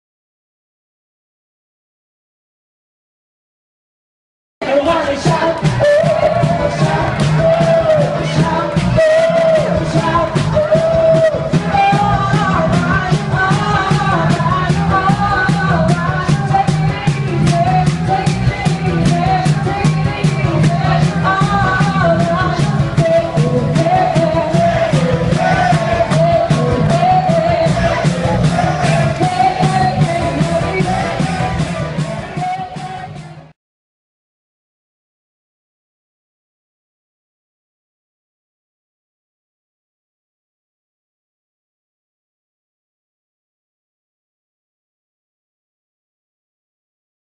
Fraggle Rock Wedding Band Ireland, New Years Eve wedding 2013
One of the best wedding bands in the west of Ireland, Fraggle Rock know how to fill a floor and rock a party!